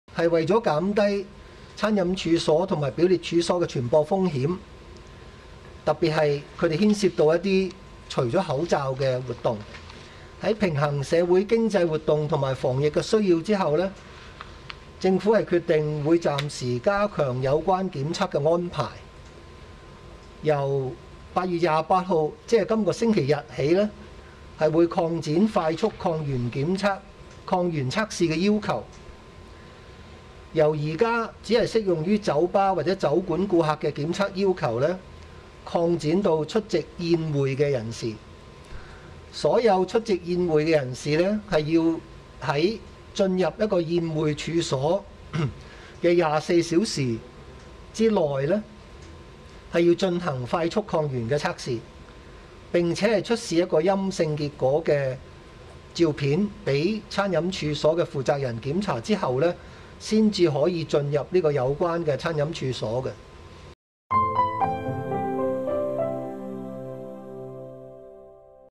香港公布抗疫新规：8月28日起出席宴会需快测
截至8月26日零时，香港新增7835宗新冠肺炎确诊病例，包括7665宗本地病例及170宗输入病例。香港特区政府医务卫生局局长卢宠茂表示，随着本地疫情持续升温，为了减低餐饮处所和表列处所的传播风险，特区政府决定由8月28日起所有出席宴会的人士，需在24小时之内进行快速抗原测试，并出示阴性结果的照片，方可进入相关的处所。卢宠茂强调，若疫情持续升温，不排除收紧其他防疫措施。（梁源 范思忆）